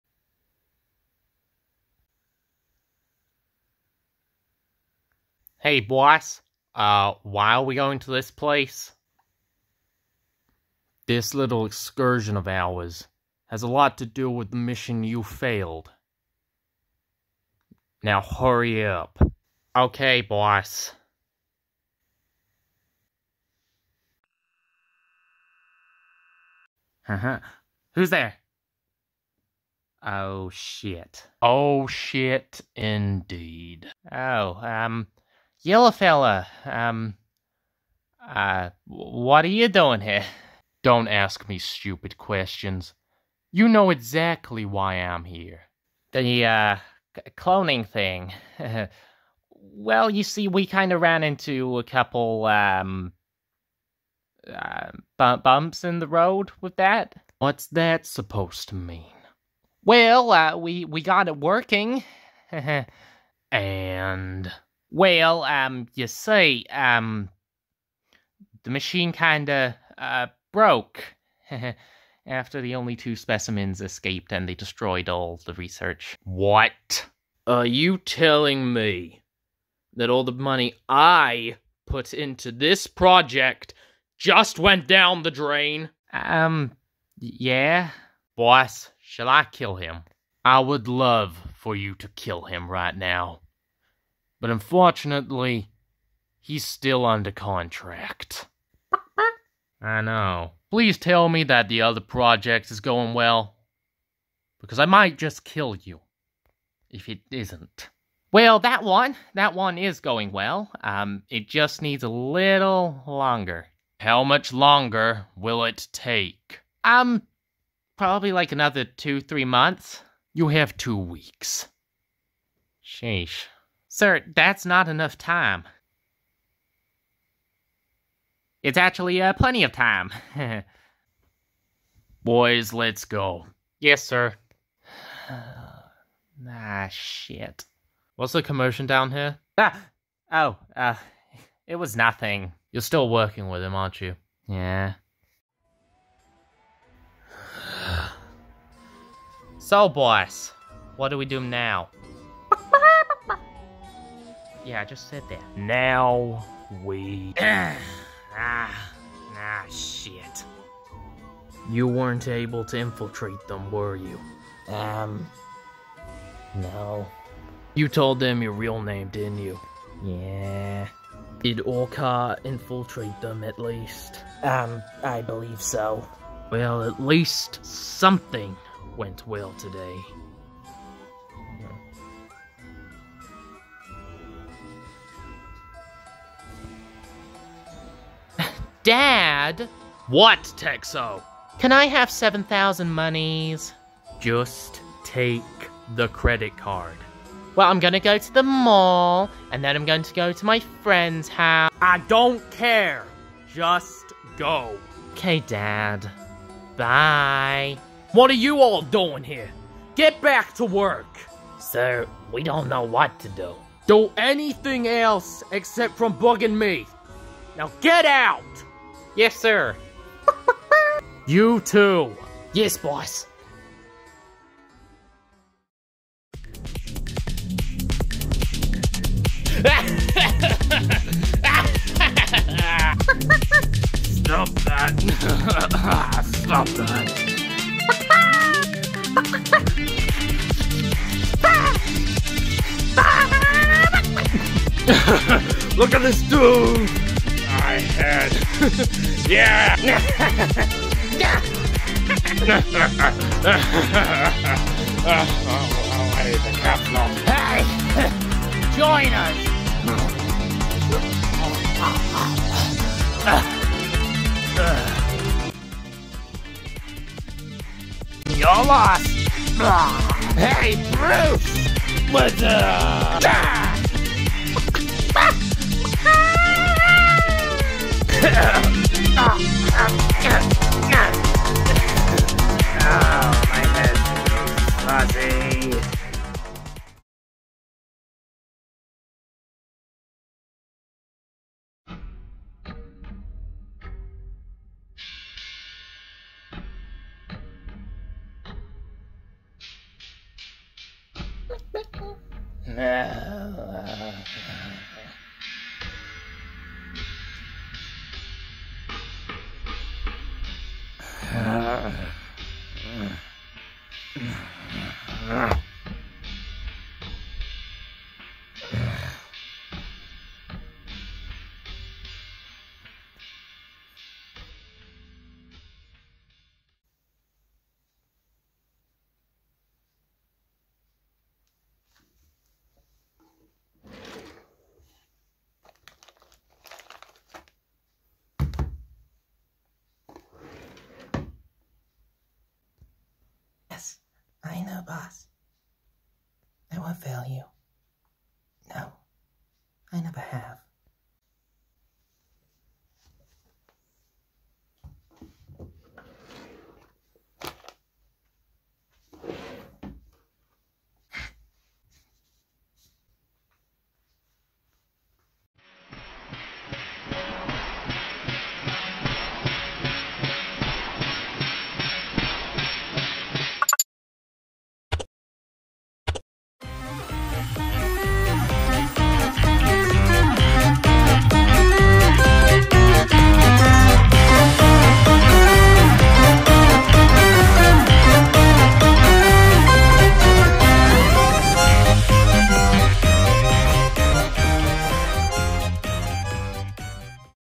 THE YELLOW-FELLOW - ANS: Liminal // S2: Episode 8
[The Yellow-Fellow and his cronies start preparing for their eventual battle with The Main Characters Tm.]

Adventures'N'Stuff: Liminal is a plush show that exists (for some reason). It's about a puppet otter and bat, who have a lot of wacky misadventures (for the most part).